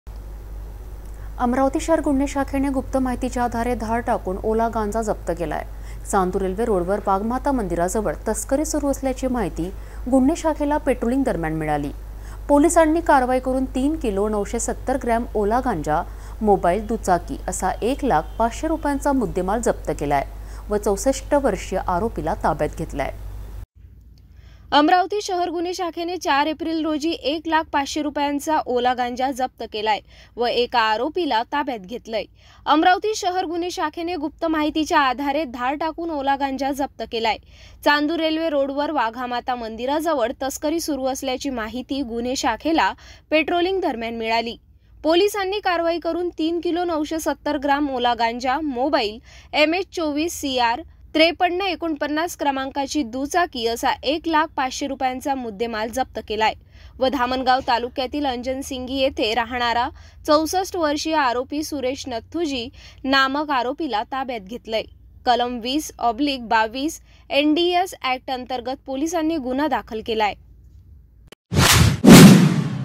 अमरावती शहर गुन्हे शाखेने ४ एप्रिल रोजी १लाख ५०० रुपयांचा ओला गांजा केला जप्त
३ किलो ९७० ग्रॅम ओला गांजाची चांदुर_रेल्वे रोडवर होत होती तस्करी
ओला गांजा, मोबाईल, दुचाकीसह ६४ वर्षीय आरोपीला गुन्हे शाखेने घेतलं ताब्यात